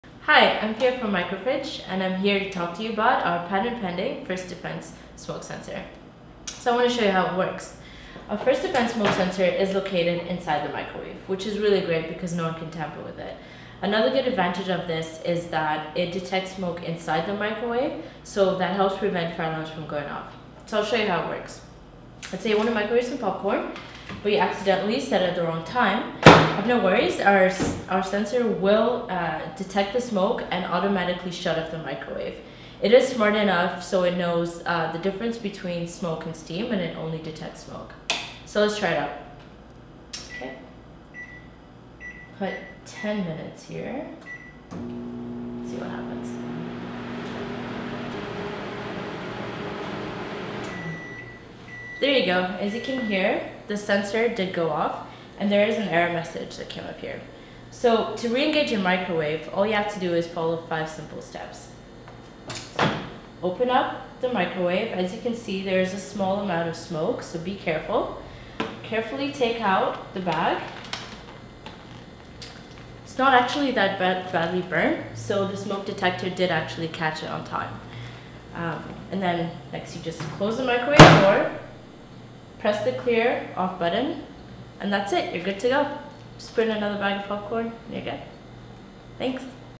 1st Defense™ Smoke Sensor Technology
What if your combination appliance helps prevent unnecessary fire alarms from going off?

Our Next Generation of MicroFridge® has a 1st Defense™ smoke sensor—an internal smoke sensor located in the microwave which detects smoke and helps to prevent fire alarms from going off.

Our new patent-pending internal smoke sensor, which is located in the microwave, automatically senses smoke, shuts down microwave, and alerts the user of smoke before any damage is done. To engage the microwave, open the microwave door, safely remove the item inside, and hit the microwave CLEAR/OFF button located on the microwave panel.